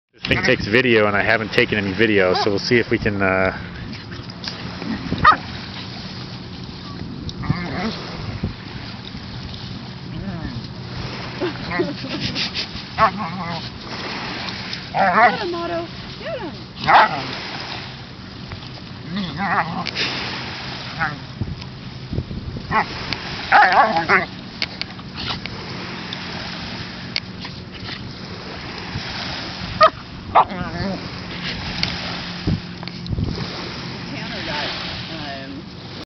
Giant Schnauzer and German Shepherd puppies playing
Otto (Giant Schnauzer puppy) playing with his friend Tanner (German Shepherd puppy) at the lagoon.